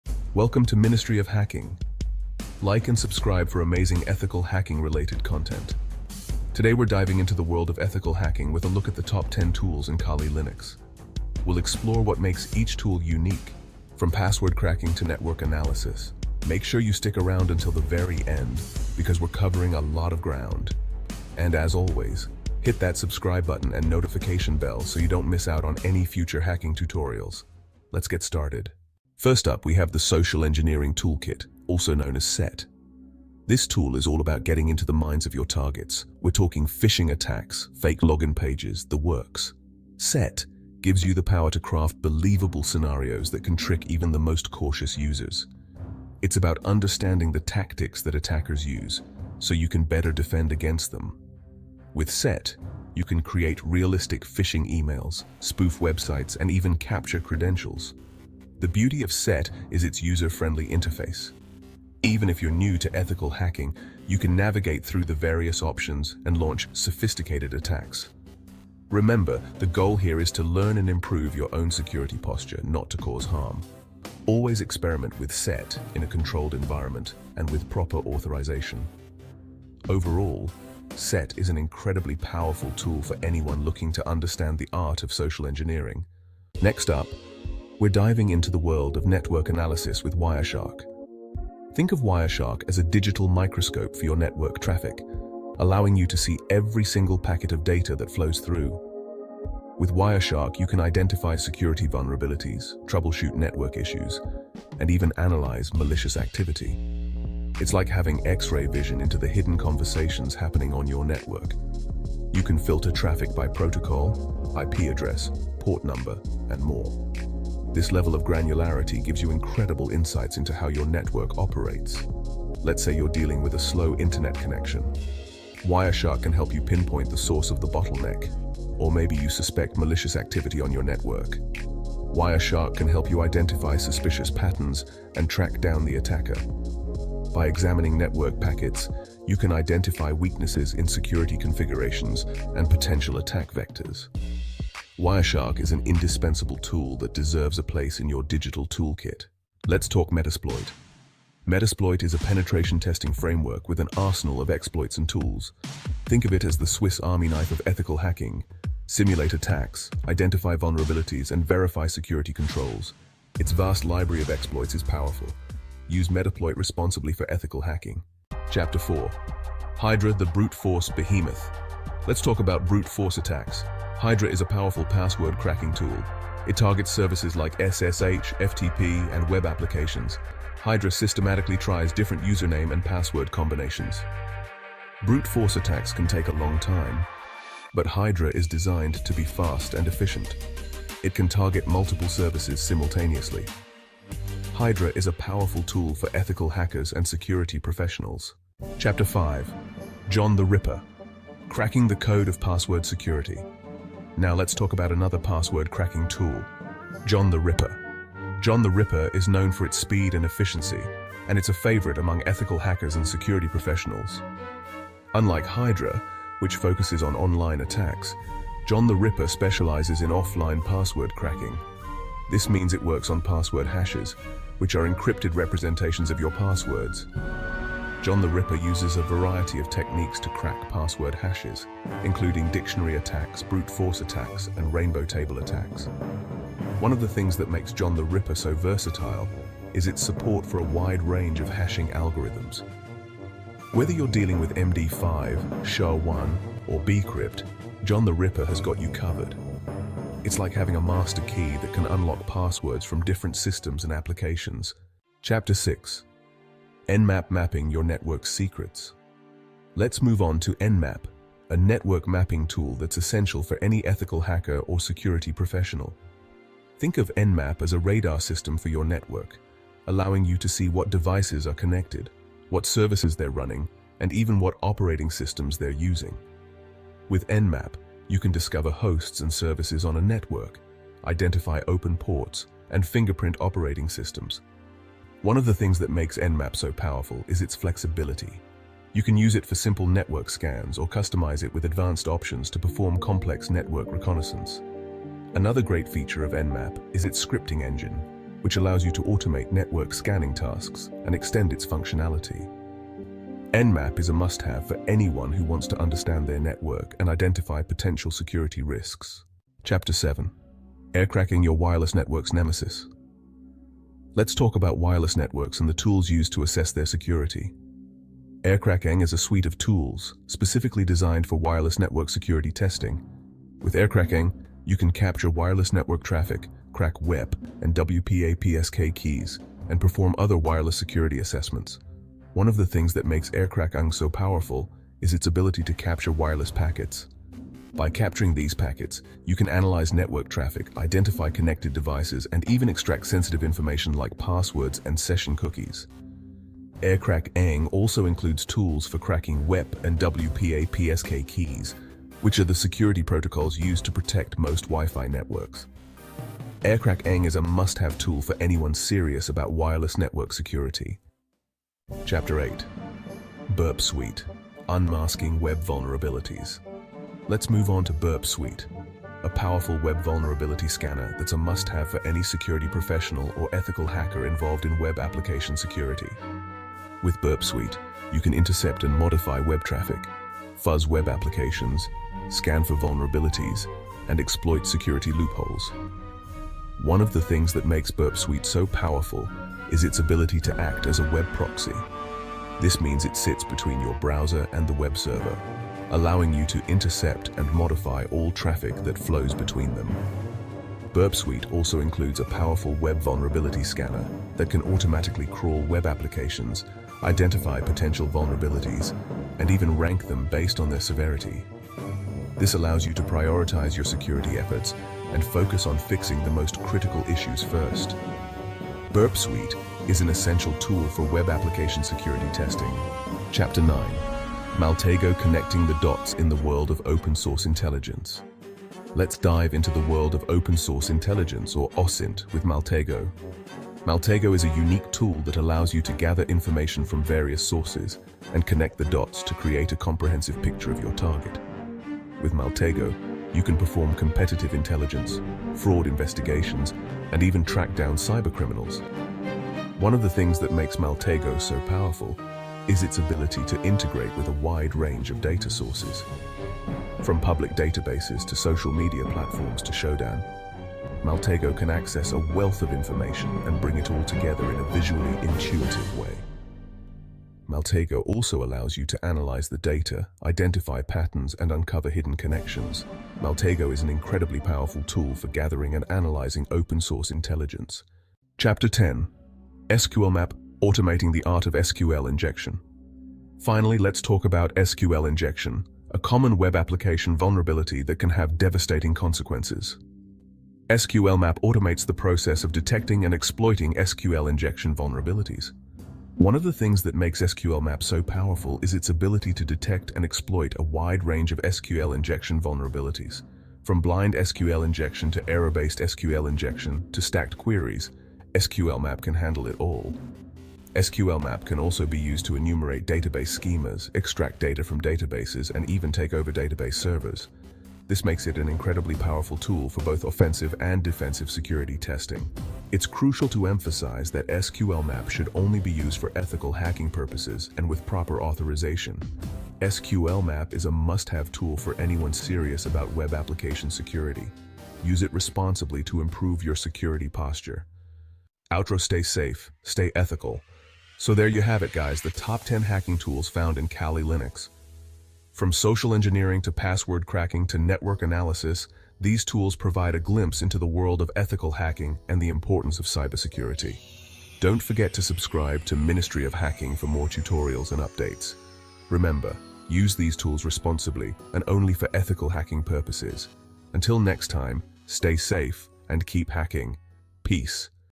Top 10 Social Media Hacking Tools in Kali Linux For Hackers
Discover the top ten tools in Kali Linux essential for ethical hacking! In this video, we'll explore powerful tools like SET, Wireshark, Metasploit, Hydra, John the Ripper, Nmap, Aircrack-ng, Burp Suite, Maltego, and SQLmap. Learn how each tool helps identify vulnerabilities, simulate attacks, and strengthen your cybersecurity skills. Whether you're a beginner or a professional, our detailed overview will guide you through the functionalities, applications, and responsible usage of these tools.

OUTLINE:

00:00:00 Welcome to the Ministry of Hacking
00:00:28 Your Key to Deception
00:01:26 Unmasking the Secrets of Your Network Traffic
00:02:29 The Ethical Hacker's Playground
00:02:48 The Brute Force Behemoth
00:03:18 Cracking the Code of Password Security
00:04:14 Mapping Your Network's Secrets
00:05:09 Your Wireless Network's Nemesis
00:06:01 Unmasking Web Vulnerabilities
00:07:03 Connecting the Dots in the World of Open-Source Intelligence
00:07:58 Automating the Art of SQL Injection
00:08:58 Stay Safe, Stay Ethical